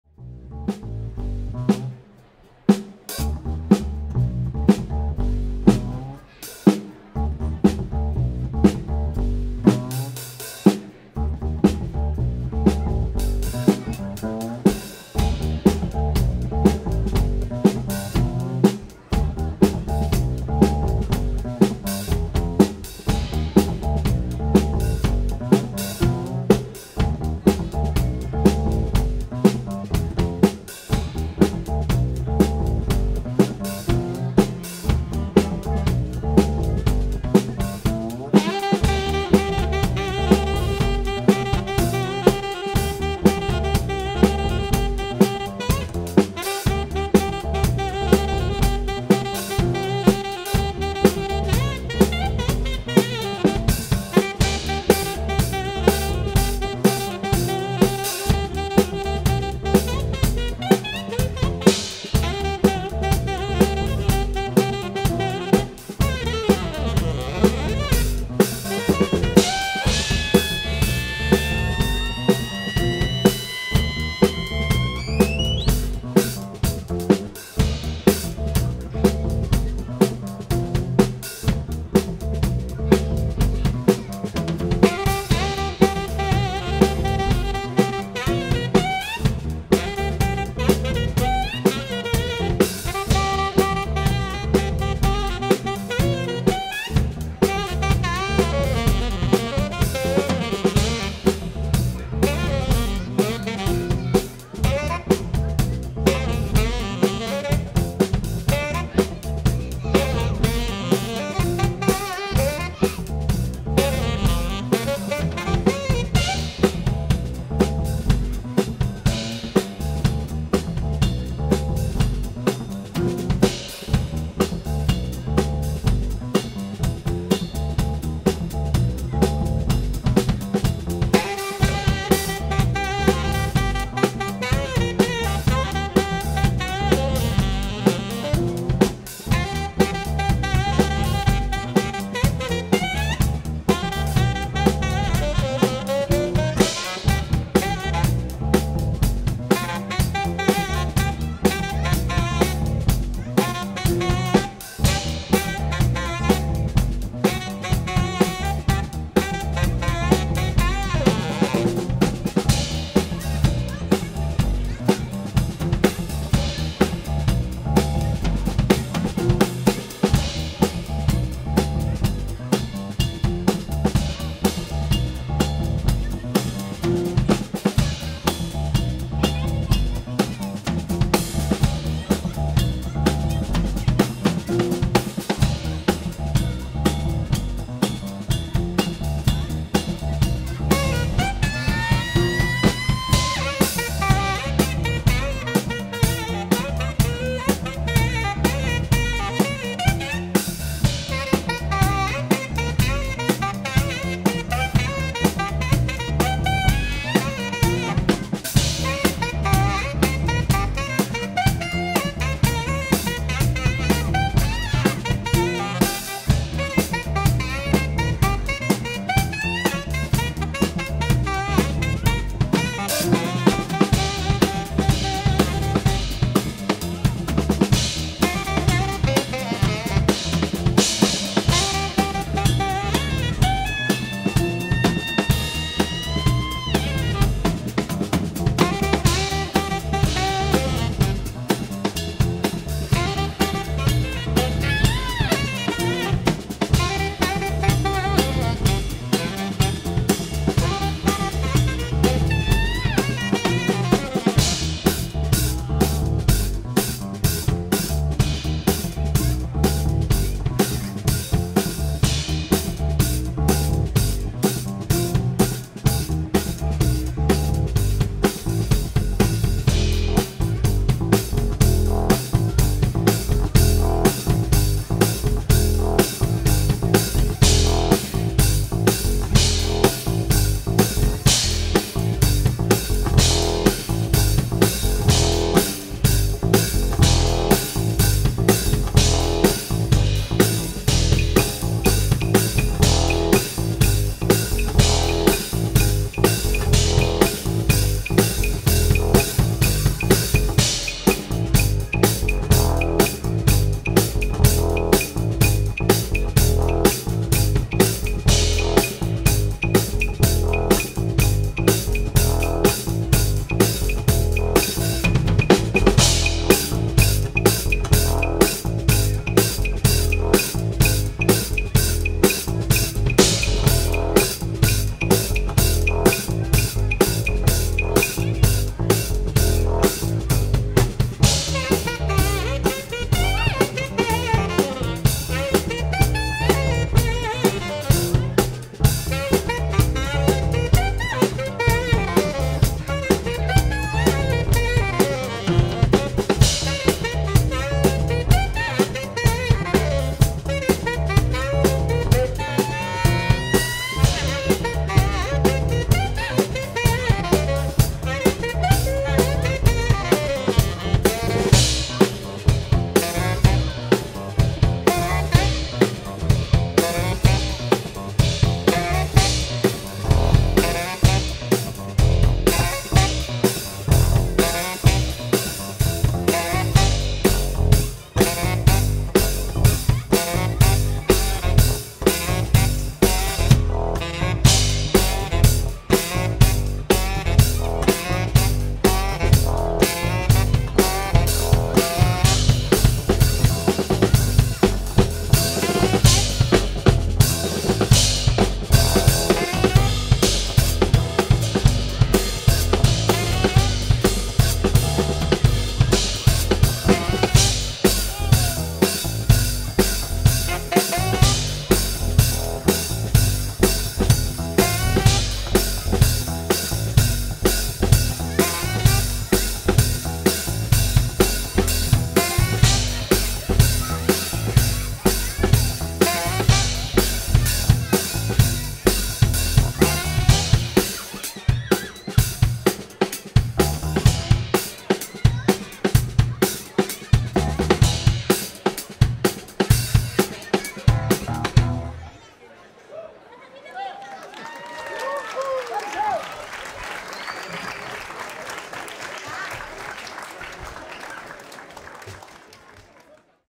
Ulrich Drechlser's AZURE / trio - Weinfest Gumpoldskirchen 2023, part 1
Ulrich Drechsler / saxophones
Oliver Steger / electric bass guitar
Raphael Keuschnigg / drums